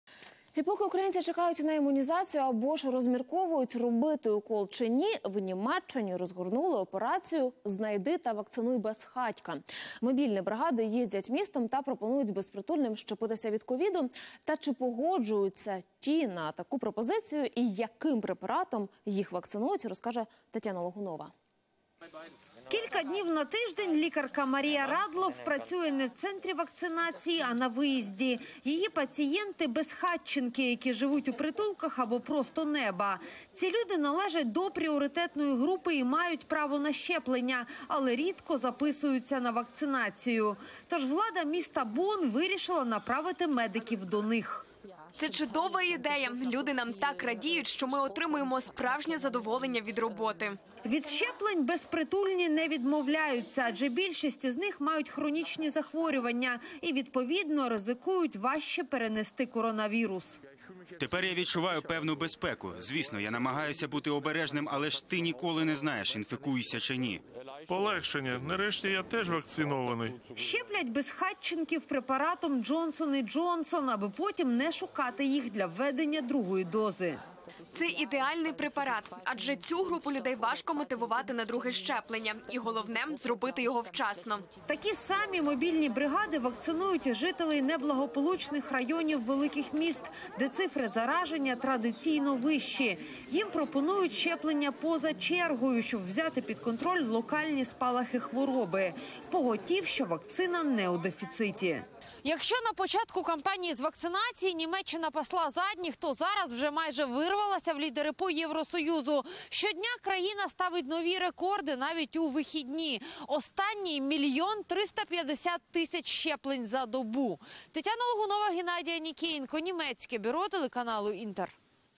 Вакцинуй безхатька: у Німеччині мобільні бригади пропонують безкоштовне щеплення безпритульним
Чи погоджуються безпритульні на пропозицію медиків і яким препаратом їх щеплюють?